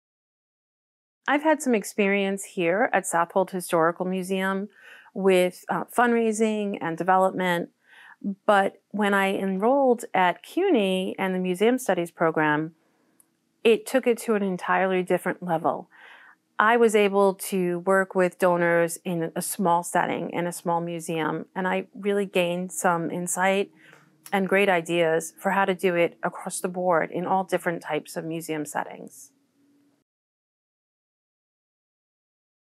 Learning to Fundraise as a Museum Studies Student | Deanna Witte-Walker | CUNY SPS
Deanna Witte-Walker discusses how she has expanded her Fundraising skills her time as a Museum Studies Student at CUNY SPS.

To learn more about the CUNY School of Professional Studies: sps.cuny.edu